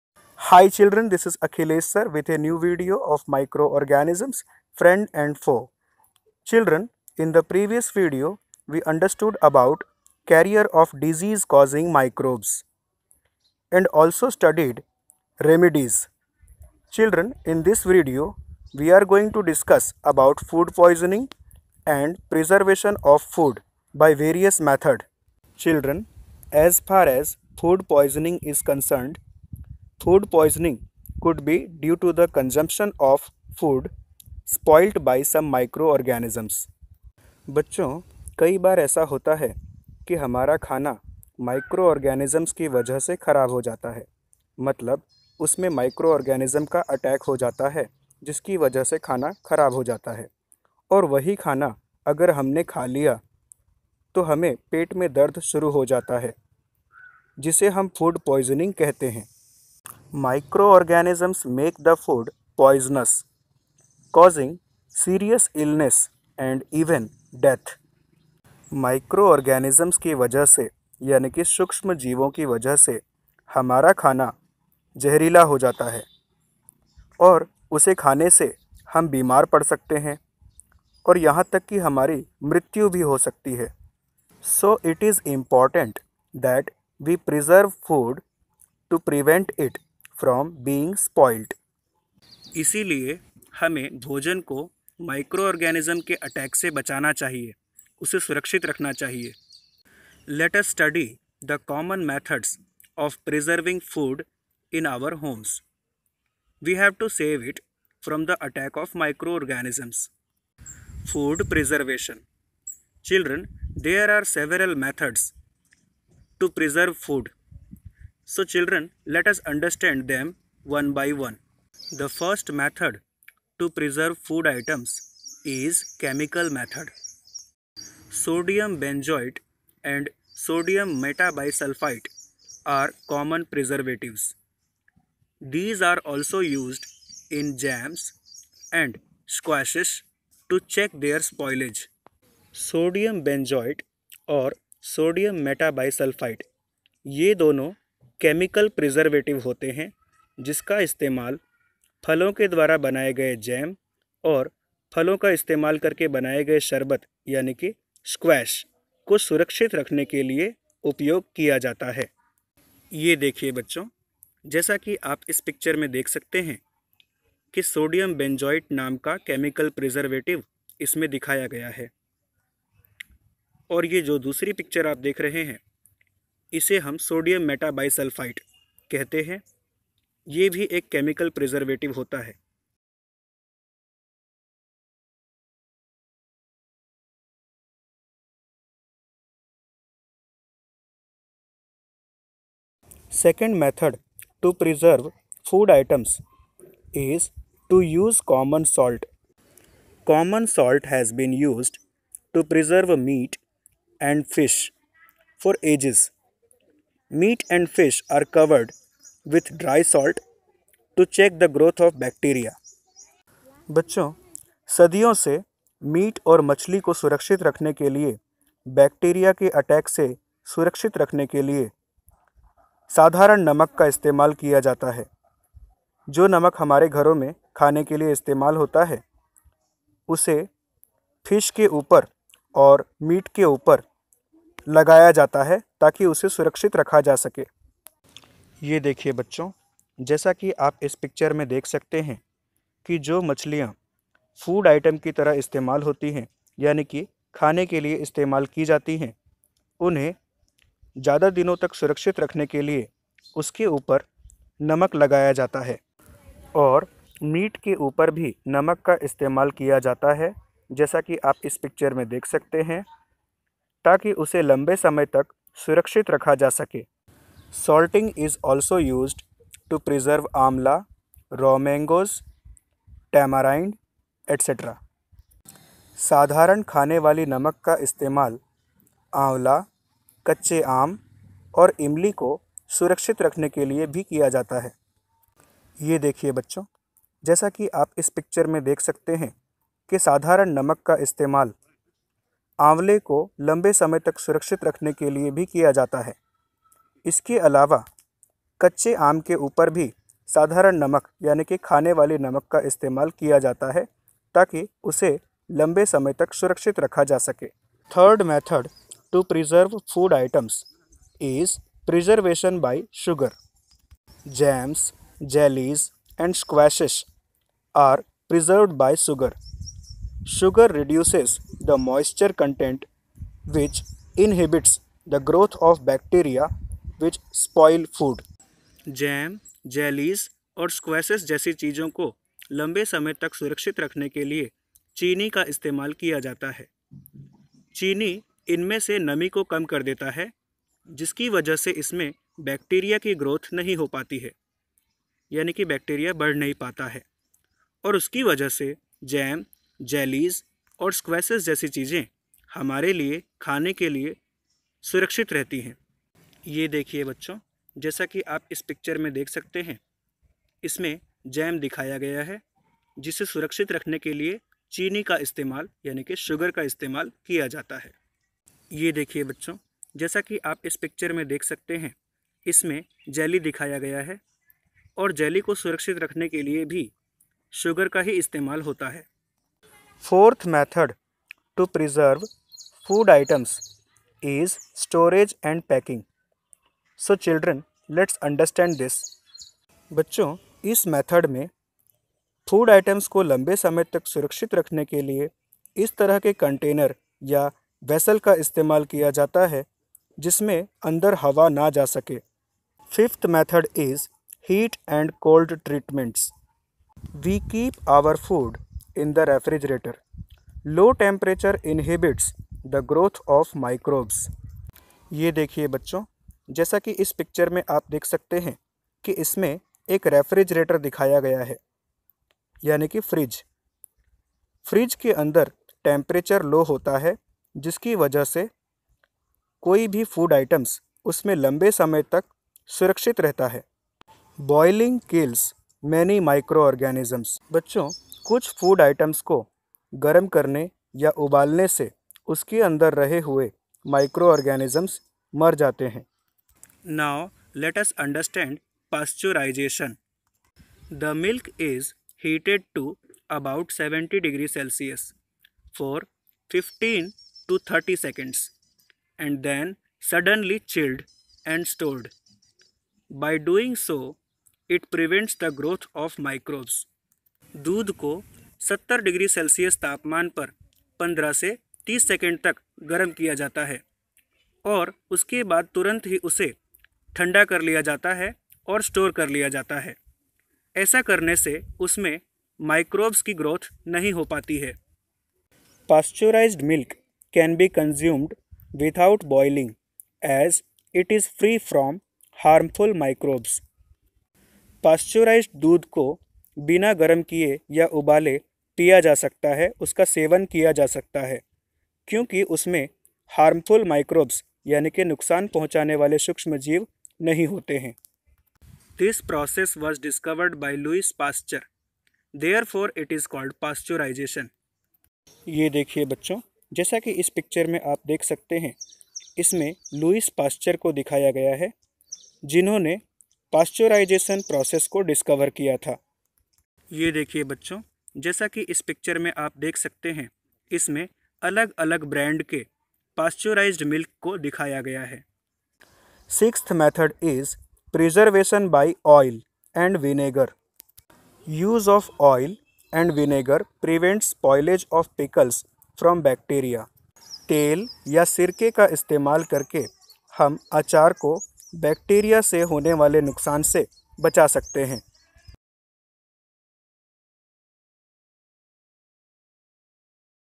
Std.8, Sub. : Science, Ch.2, Microorganisms Friends & Foe
By this video students will be able to understand about food poisoning and about food preservation methods.